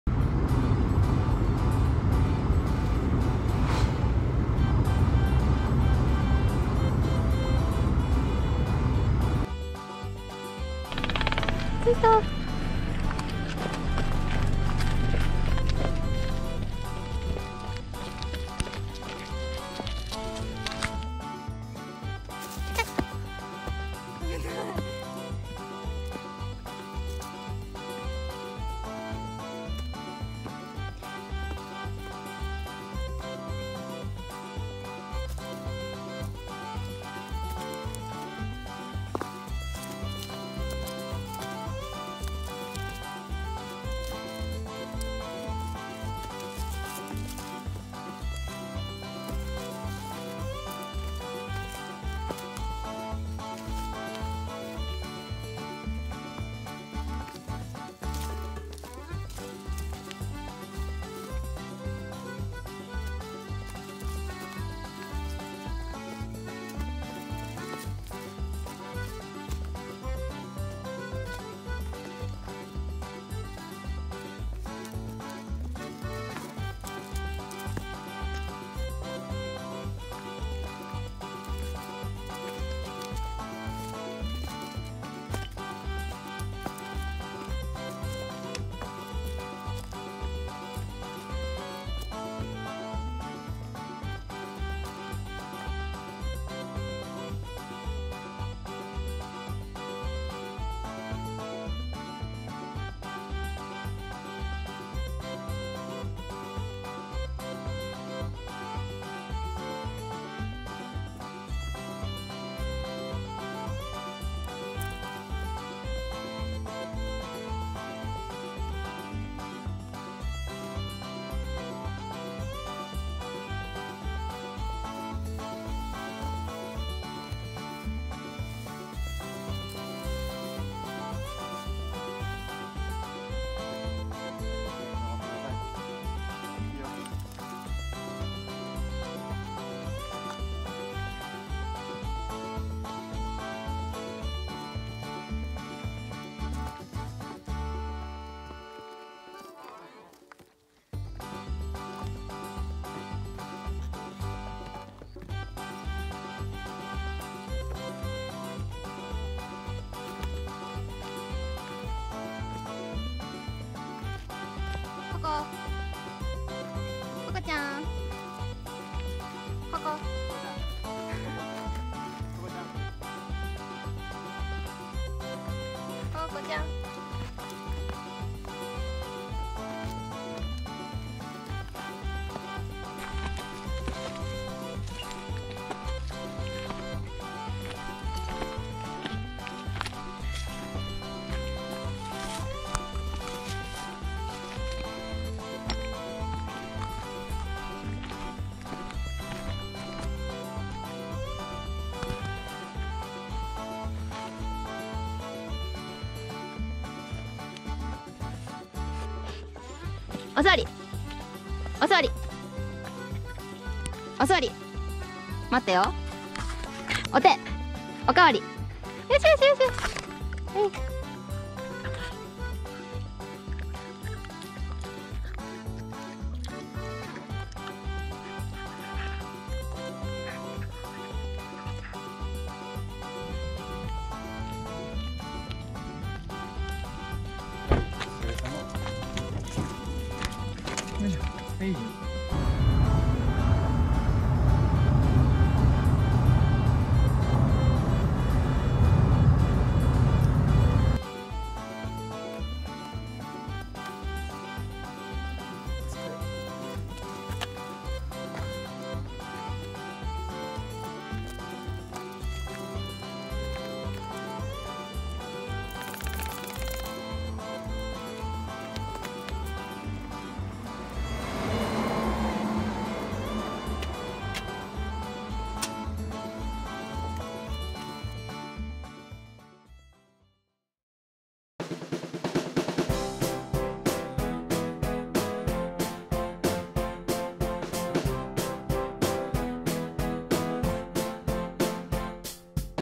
Jack Russell Terrier's Coco trying to climb a mountain
【PetsInformation】
NAME：COCO
TYPE：Jack Russell Terrier
AGE : 2 years old ♀
This channel is all about cute animals mainly dogs and cats!